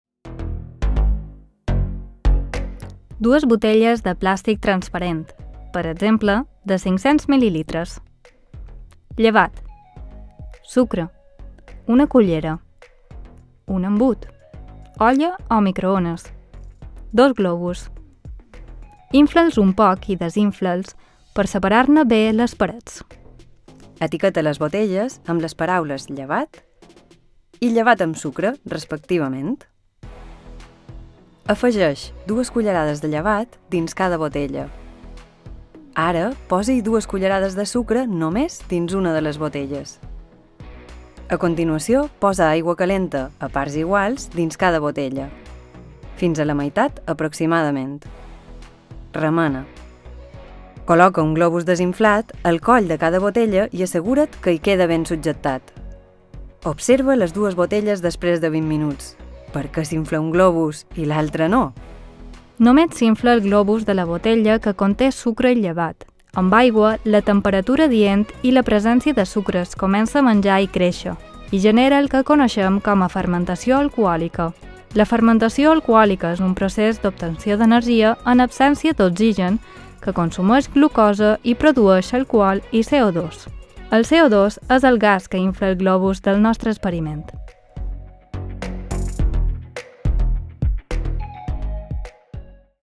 Llevat que infla globus
Són experiments senzills pensats perquè els facin els alumnes de primària a ca seva i amb materials que segur que tenen a l'abast. Els experiments que presentam ajudaran als infants a comprendre molts de fenòmens que observam en el dia a dia.